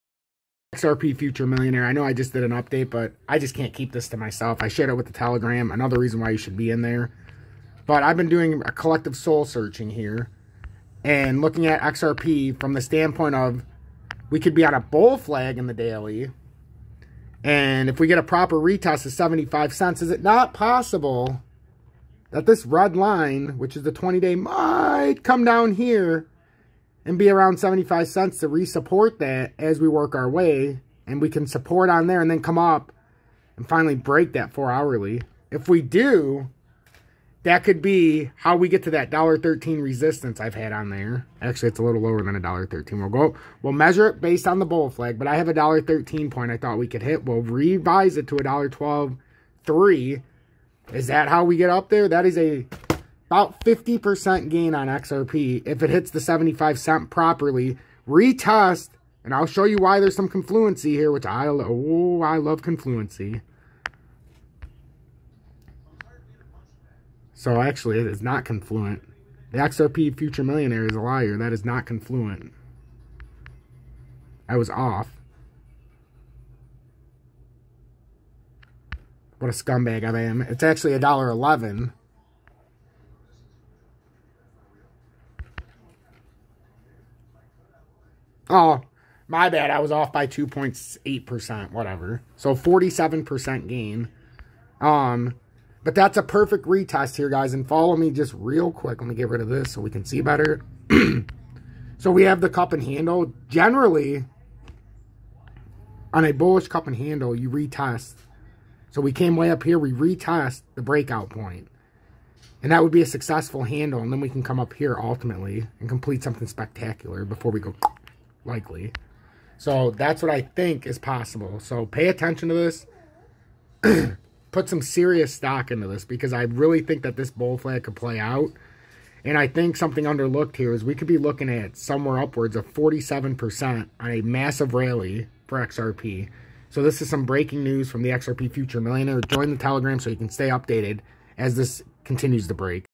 XRP BREAKING NEWS 47% PUMP PLAYING OUT IF WE RETEST 75 CENT CUP AND HANDLE BREAKOUT POINT UNREAL :)
rMdG3ju8pgyVh29ELPWaDuA74CpWW6Fxns

rMdG3ju8pgyVh29ELPWaDuA74CpWW6Fxns

rMdG3ju8pgyVh29ELPWaDuA74CpWW6Fxns